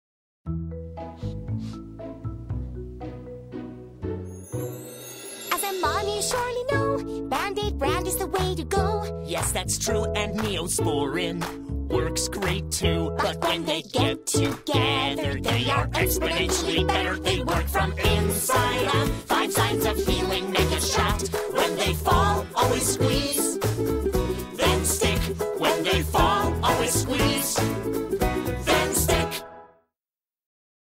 Band-Aid/Neosporin advertisement (2010)
"Always squeeze, then stick!" This recently-created muppet-esque advertisement shows the importance of combining Johnson & Johnson's antibiotic treatment with their bandages.

EDIT 03/01/11: Corrected the ending lyrics that introduces this video in the doobly-doo. Hope you've enjoyed the ad!

EDIT 06/09/11: 50k views! It has certainly been a pleasure to read your comments on this video. Nothing like this anyway. Nonetheless, I thank you all who have came here and liked this video.